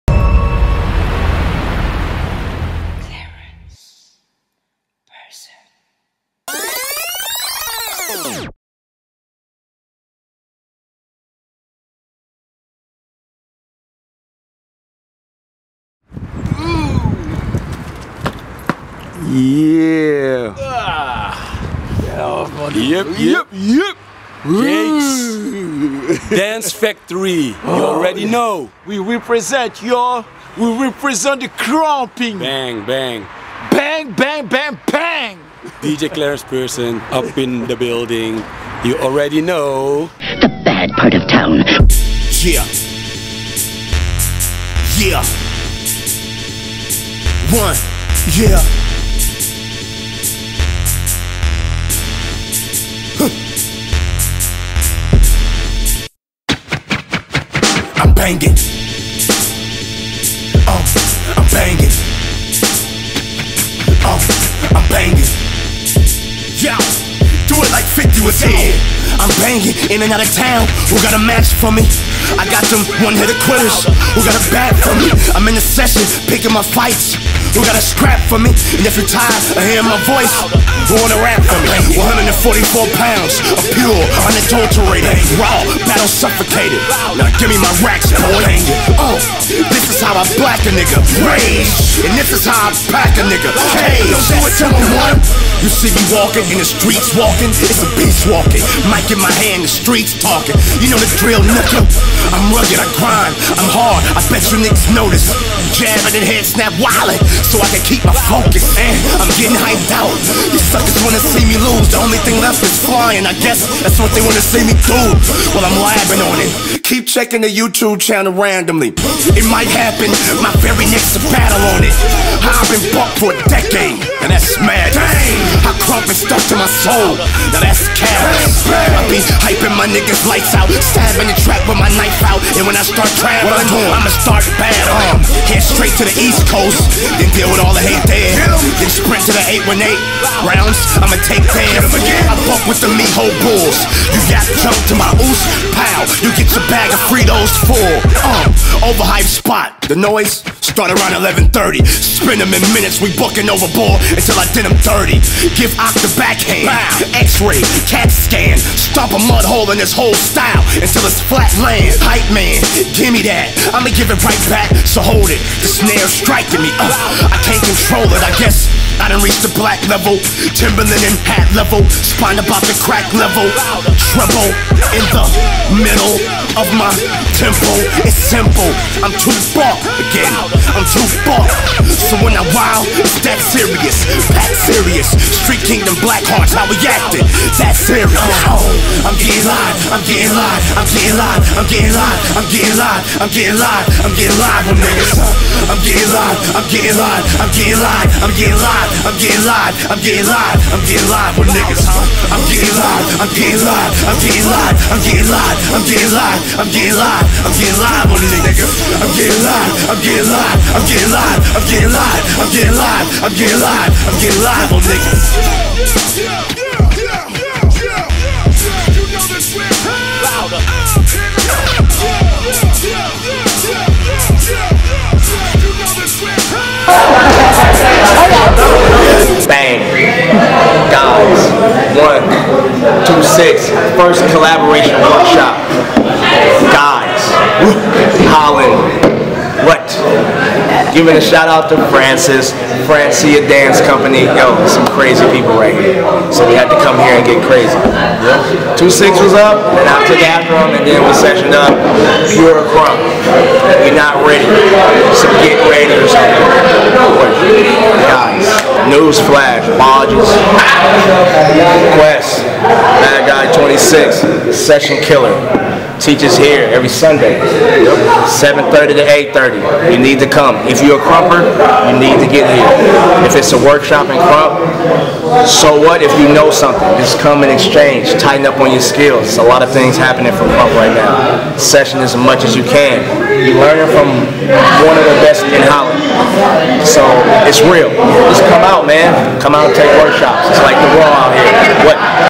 BaddGuyz Collabo Workshop Tight Eyez & Ques | Recap by Claerence Person | FDC Dance Company HD
just watch and njoy
some dope shots of the workshops
more projects will come
allround artist